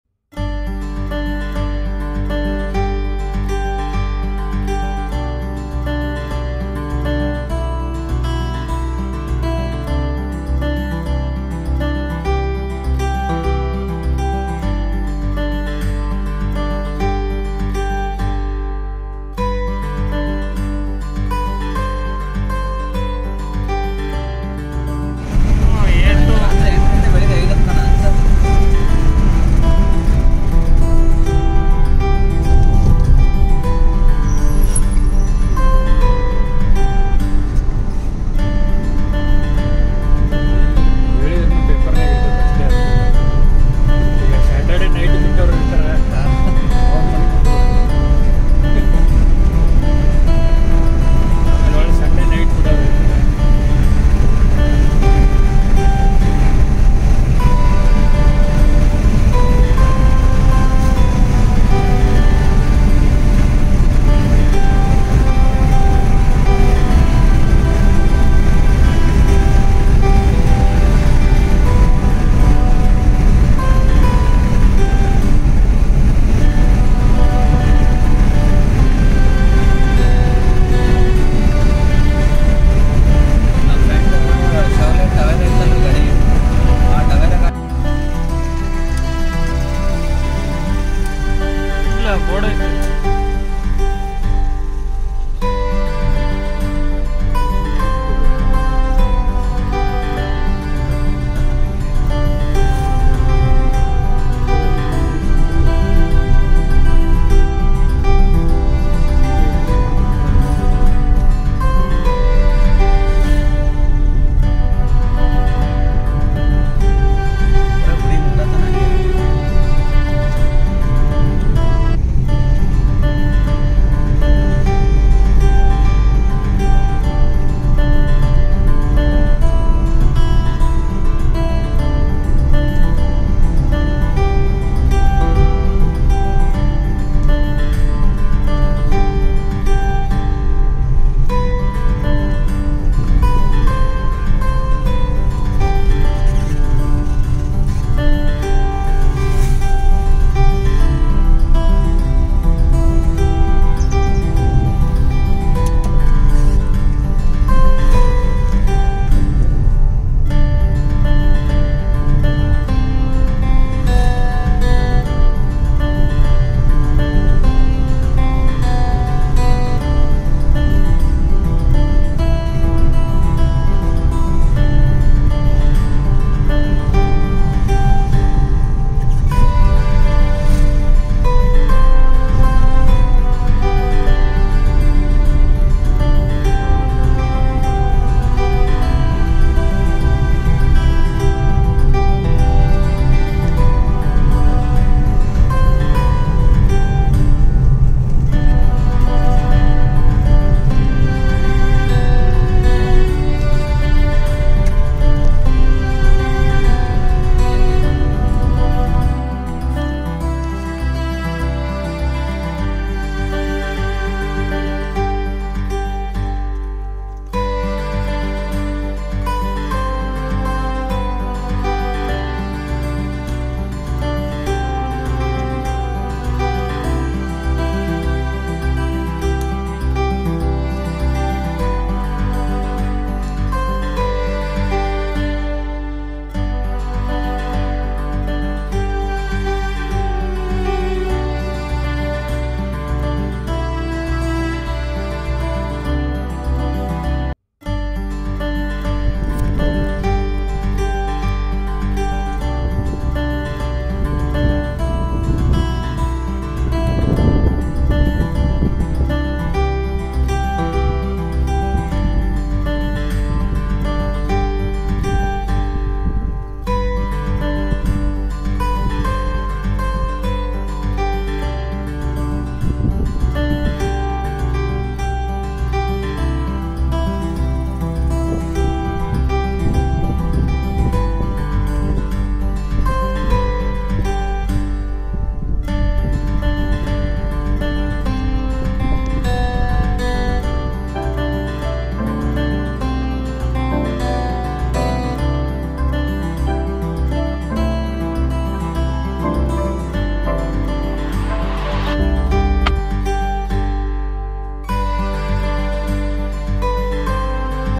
Road Trip to Kailashgiri from Bangalore
Kailashgiri is one of most pilgrim place  in kolar. Temple has built inside the rock hill, making completely as cave. The place is surrounded by beautiful series of hills, greenery lands and lakes.
If you are looking for a comfortable one day trip from Bangalore, kailashgiri is much more package than suitable. This place gives most attractive hill spots, garden, trecking, so on. A complete family day-off spot. We encourage you to explore the rich landscape, heritage and beautiful spots surrounding Bangalore.